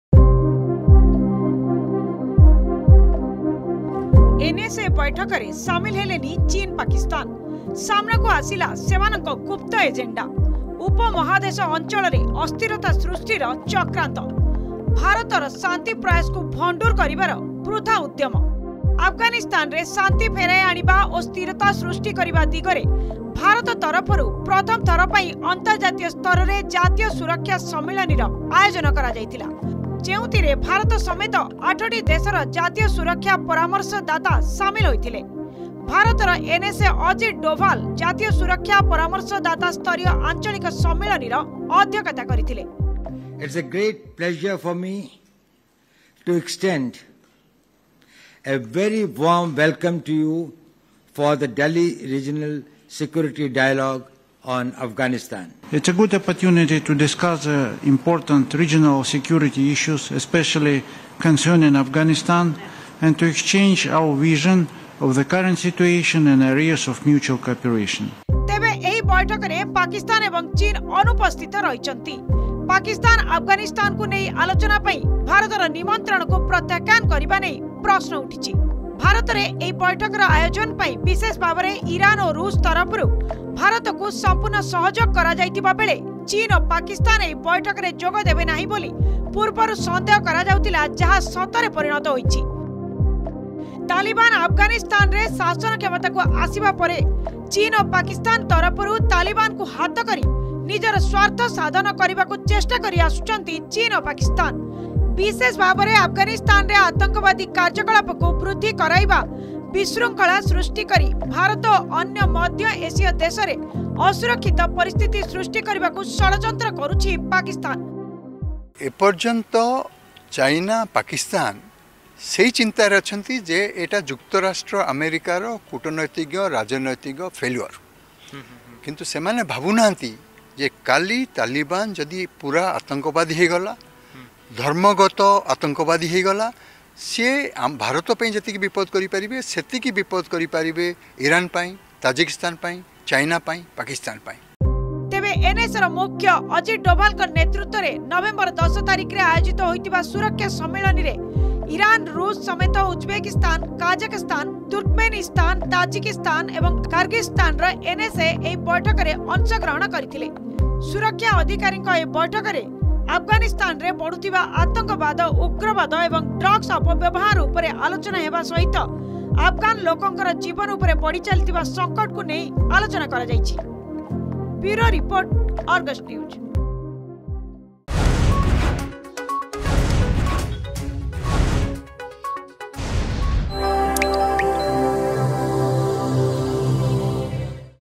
ଏନଏସଏ ବୈଠକରେ ସାମିଲ ହେଲେନି ଚୀନ ପାକିସ୍ଥାନ
ସାମନାକୁ ଆସିଲା ସେମାନଙ୍କ ଗୁପ୍ତ ଏଜେଣ୍ଡା
ଉପମହାଦେଶ  ଅଂଚଳରେ ଅସ୍ଥିରତା ସୃଷ୍ଟିର ଚକ୍ରାନ୍ତ
ଆଫଗାନିସ୍ତାନରେ ଶାନ୍ତି ଫେରାଇ ଆଣିବା ଓ ସ୍ଥିରତା ସୃଷ୍ଟି କରିବା ପାଇଁ ଭାରତ ତରଫରୁ ଦିଲ୍ଲୀରେ ପ୍ରଥମ ଥର ପାଇଁ ଅନ୍ତର୍ଜାତୀୟ ସ୍ତରରେ ଜାତୀୟ ସୁରକ୍ଷା ସମ୍ମିଳନୀର ଆଯୋଜନ କରାଜାଇଥିଲା ।  ଯେଉଁଥିରେ ଭାରତ ସମେତ 8ଟି ଦେଶର ଜାତୀୟ ସୁରକ୍ଷା ପରାମର୍ଶଦାତା (NSA) ସାମିଲ ହୋଇଥିଲେ । ତେବେ ବୈଠକରେ ସାମିଲ ହୋଇ ନଥିବା ଚୀନ ଓ ପାକିସ୍ଥାନ ଜାହାକୁ ନେଇ ପ୍ରଶ୍ନ ଉଠିଛି ।